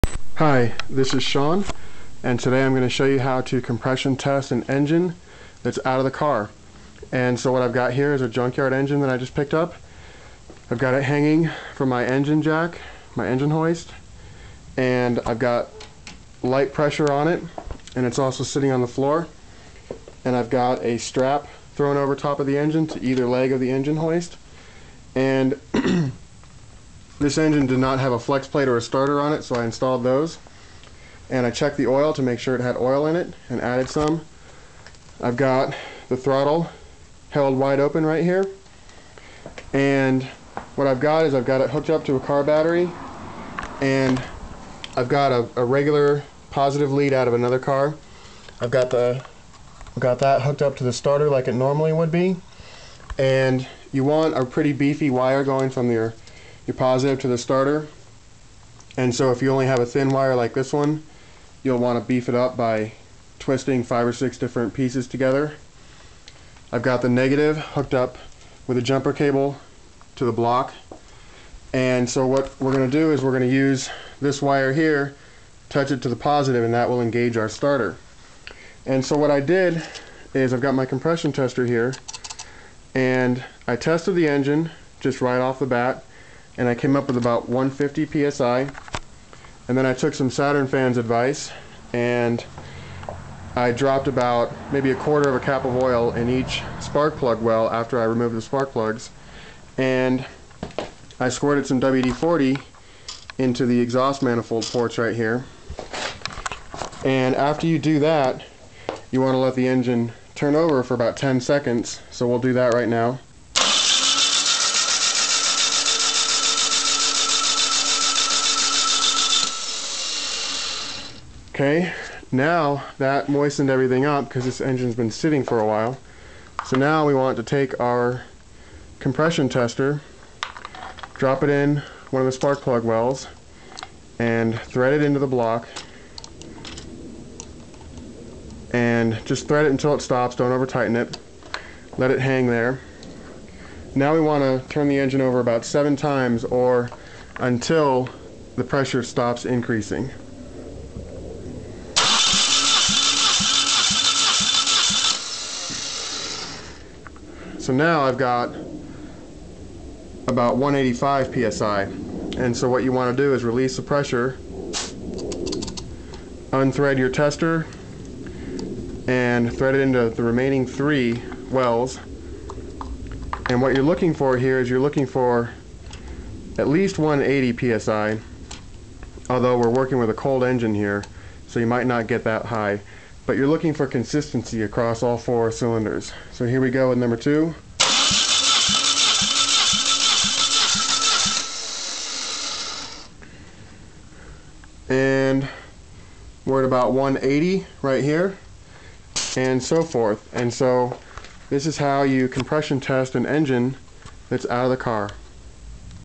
Saturn compression test.AVI
Compression test a saturn engine out of the car, shawn villalpando